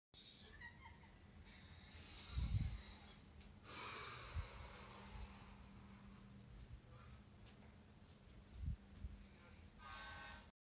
X-Mas Dalmation Dog Looks Around My Bedroom
A video of the X-Mas dalmation dog looking around my bedroom.  Alright so this was a test video, because I'm trying out a new tour of my bedroom.